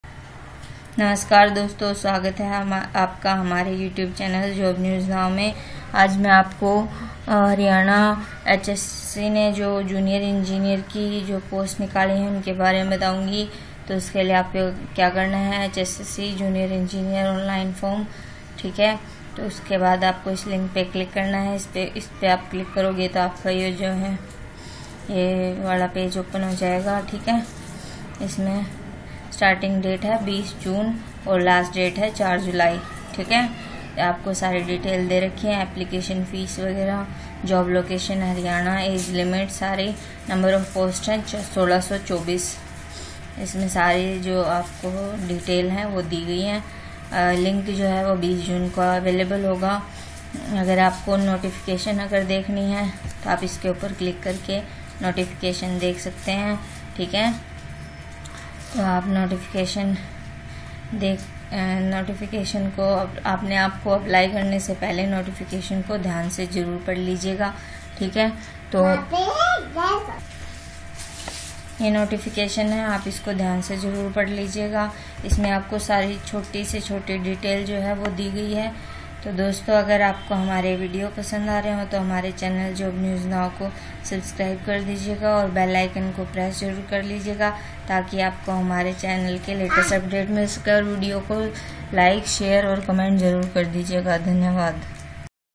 HSSC JUNIOR ENGINEER POST ONLINE FORM 2019
Hello friends

Vacancy Details for HSSC JE Recruitment  Online Form 2019

Post Name – Junior Engineer(Electrical/Civil/Mechanical/Horticulture)